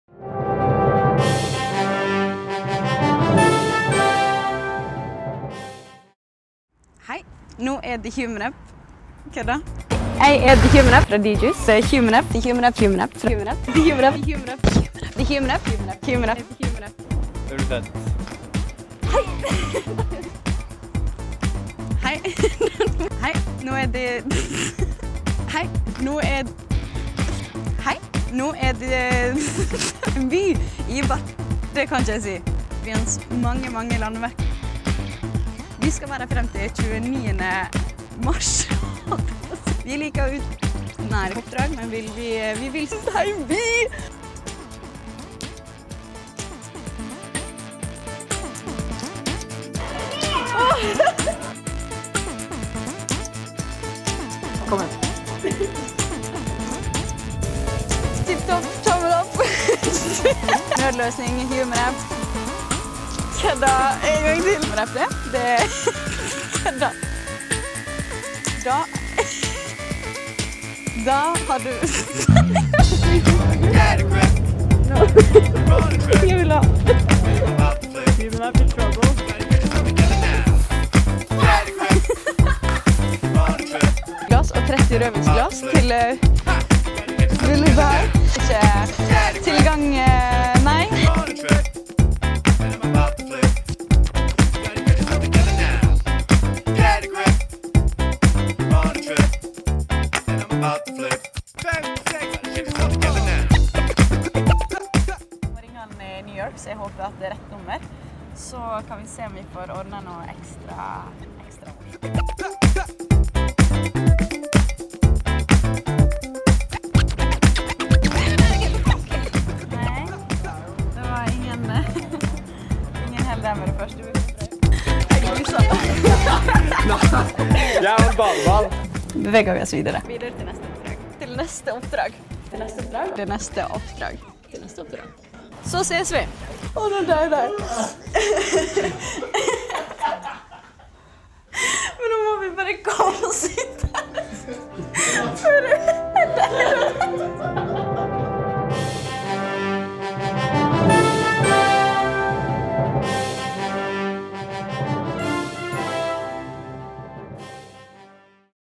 Behind the scenes
Her kan du se upubliserte klipp, tabber og morsomme episoder fra The Human App-turneen. Fortsett å følge djuice på Facebook og få informasjon om flere aktiviteter i fremtiden.